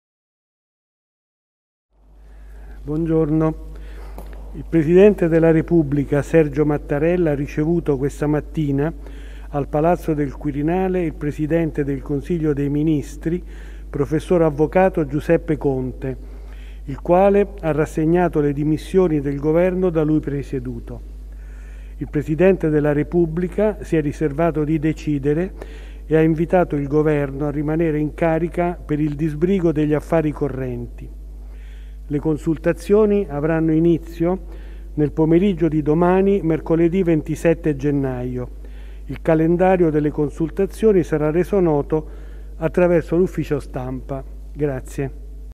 Quirinale, il segretario Zampetti annuncia le dimissioni di Conte
La comunicazione ufficiale del Colle in attesa del calendario delle consultazioni

POLITICA (Roma). Il segretario generale del Quirinale, Ugo Zampetti, ha annunciato alla stampa che il presidente del Consiglio, Giuseppe Conte, ha rassegnato le proprie dimissioni da capo dell'esecutivo nelle mani del presidente della Repubblica. Zampetti ha anche precisato che il calendario delle consultazioni, che inizieranno domani, non è stato ancora definito. (a.mariani)